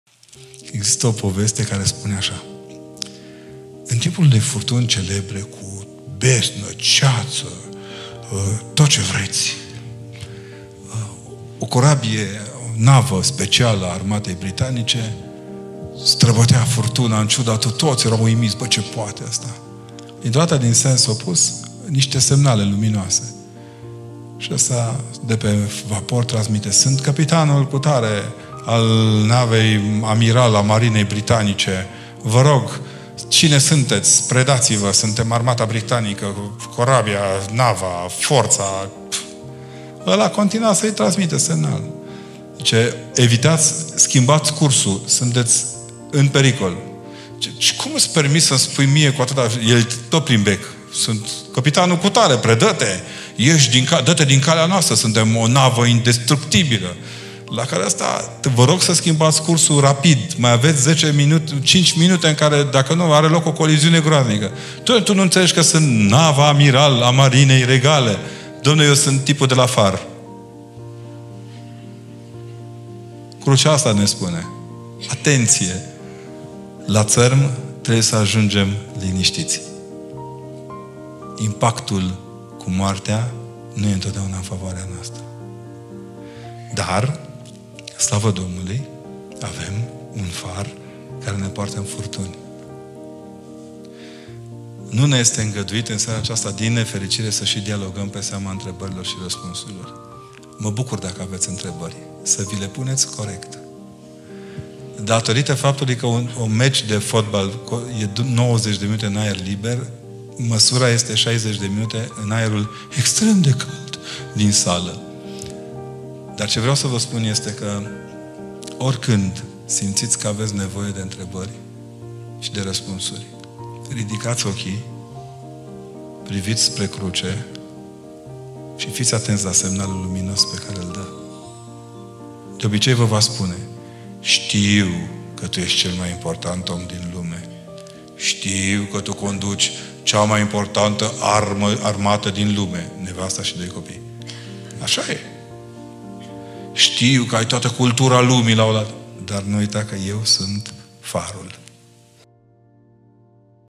„Crucea – farul nostru care ne poartă în furtuni” – Pr. Constantin Necula
Fragment din conferința „Crucea libertății duhovnicești”, susținută de Părintele Constantin Necula duminică, 4 aprilie 2021, la Casa de cultură „Ion Creangă” din Târgu Neamț